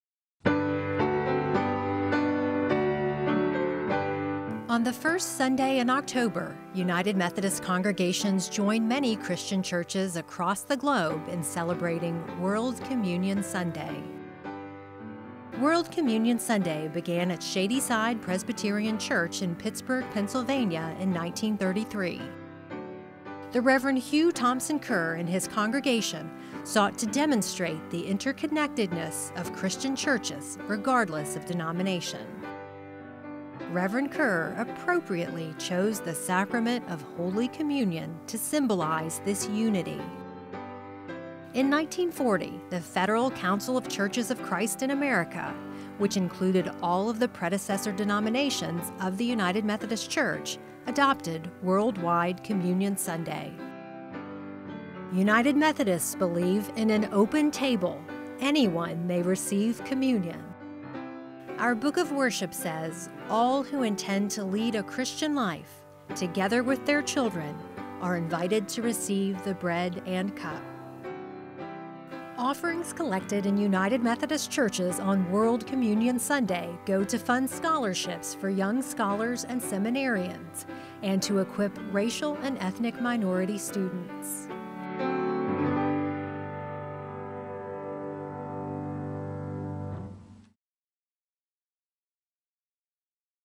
United Methodist World Communion Sunday
The first Sunday in October is World Communion Sunday. United Methodist congregations join many Christian churches across the globe for this celebration of unity among churches across denominations. United Methodist Churches collect a special offering on World Communion Sunday to fund scholarships for young scholars and seminarians and to equip racial and ethnic minority students.